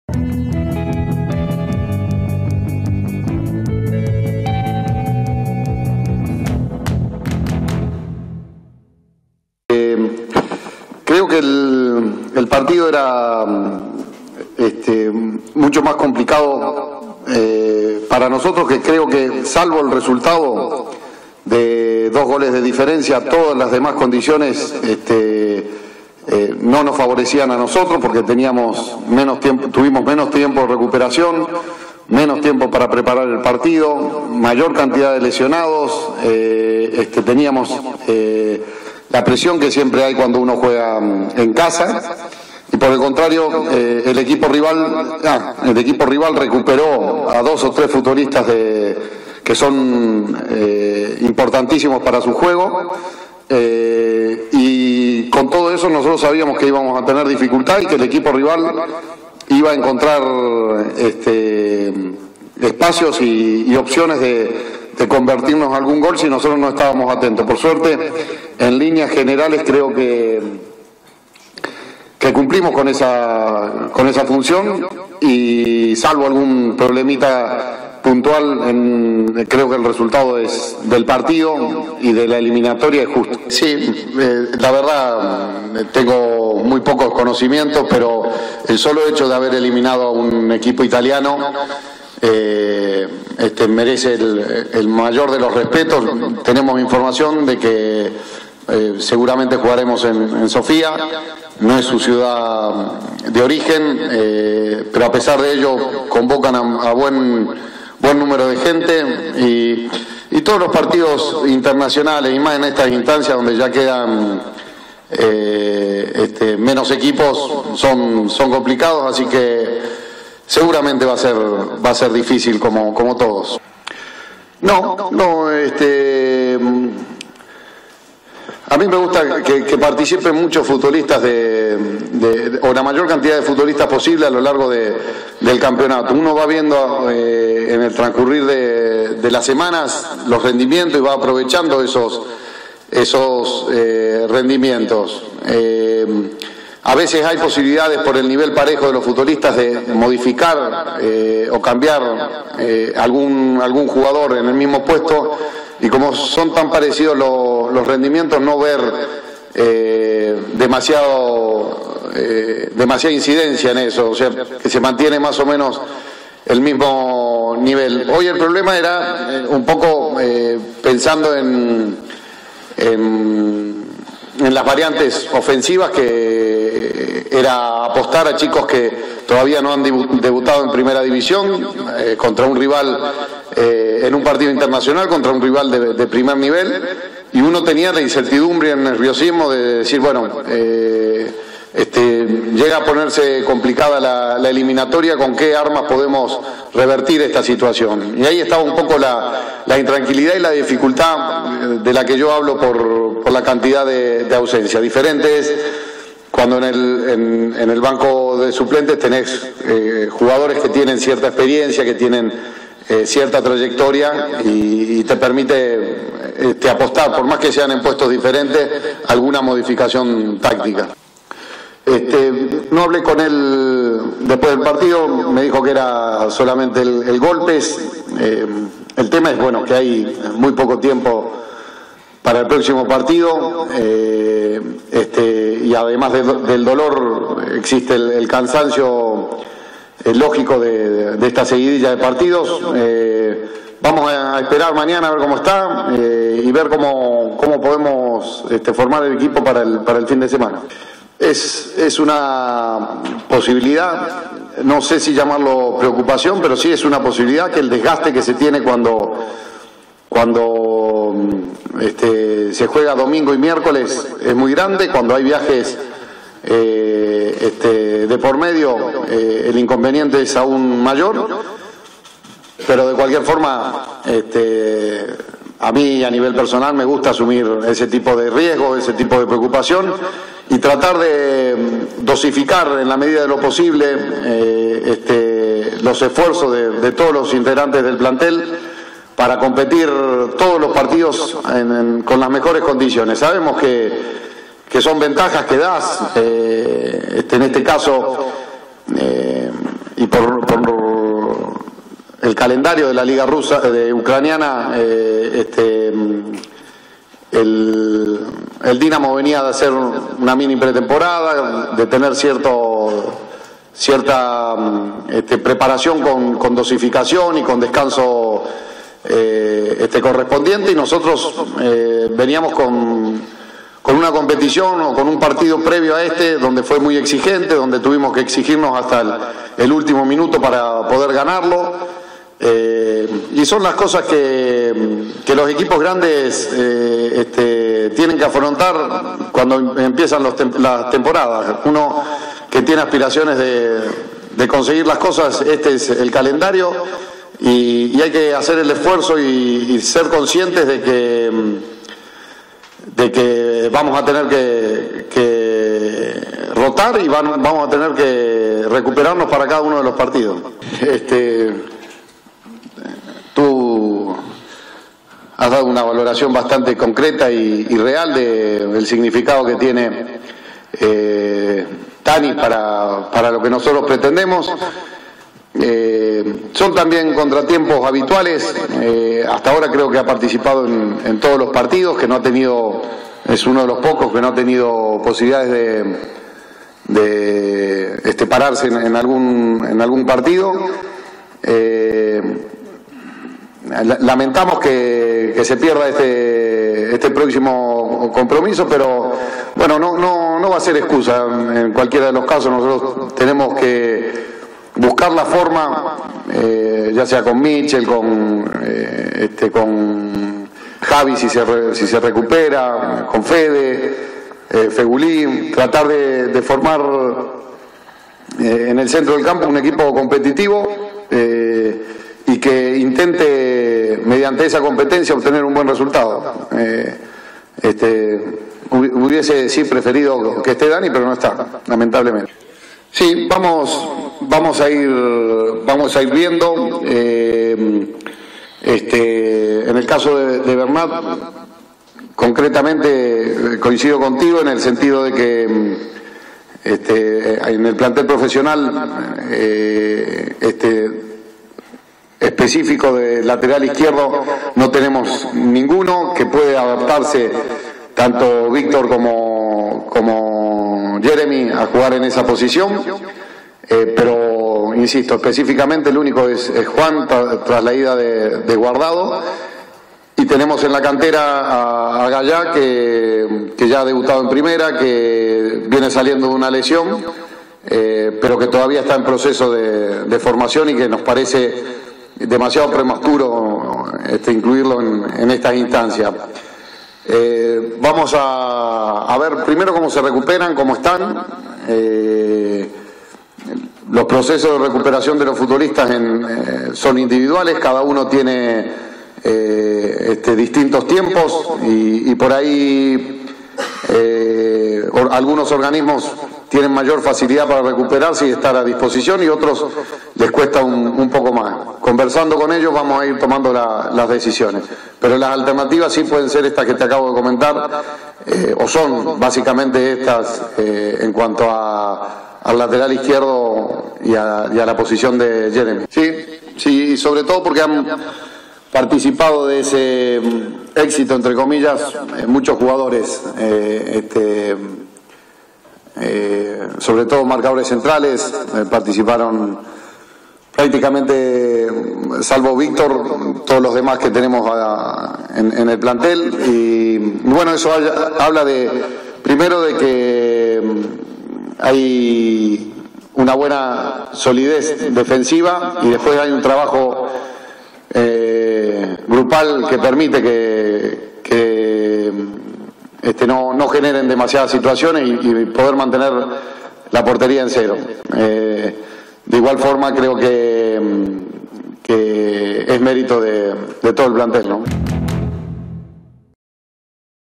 Rueda de prensa de Pizzi post partido contra el Dinamo
El entrenador argentino ha valorado el pase a octavos de final de su equipo tras empatar a cero en Mestalla contra el Dinamo